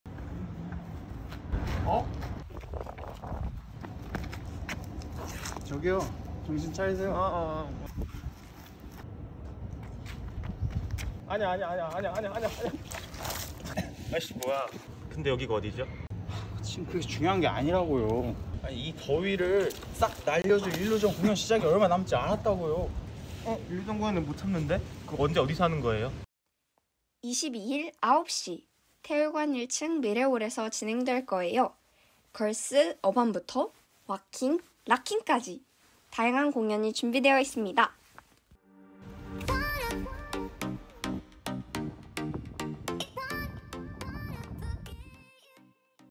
[22봄 정기공연] 홍보영상
2022 ILLUSION 봄학기 정기공연 홍보영상입니다.

출연 : 박민규, 윤종범
촬영 : 이혜리
편집 : 차영주